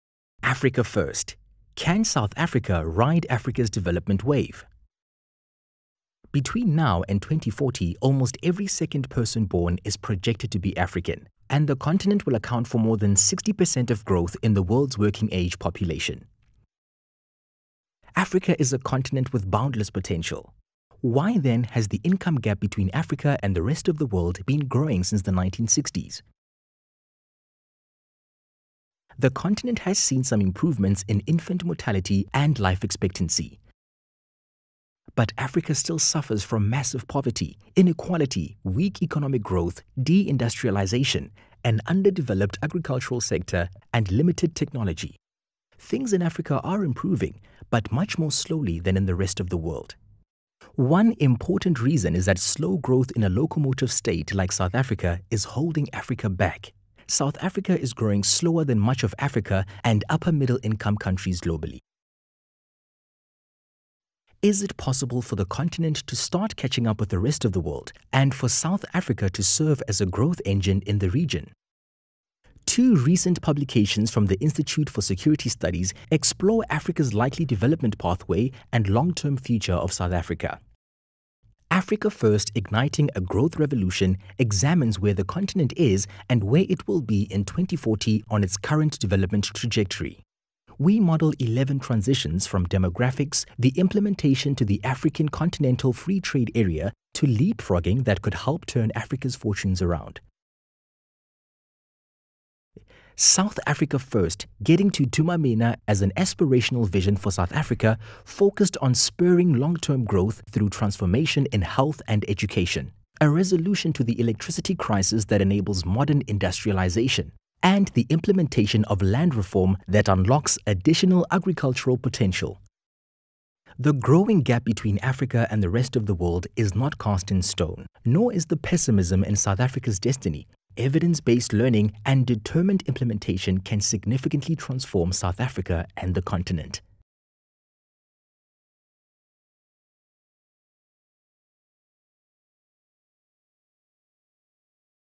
Africa First! Can South Africa ride Africa’s development wave?
The economic growth gap between Africa and the rest of the world isn't cast in stone.